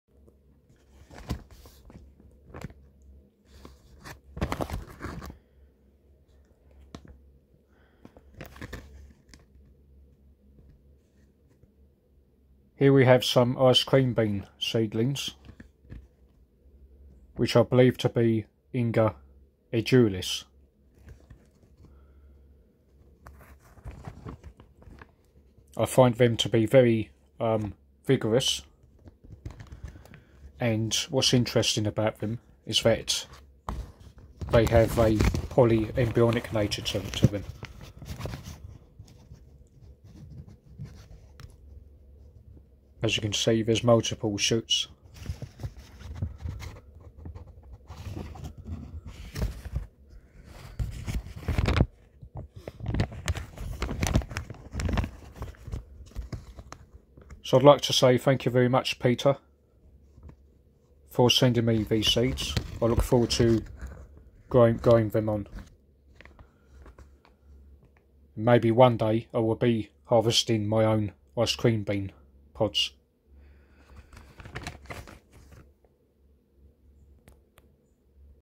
Polyembryonic Inga Sp. seedlings
Probably Inga edulis. I. edulis and other Inga species are commonly known as 'Ice Cream Bean'.

These seedlings have a lot of vigor to them.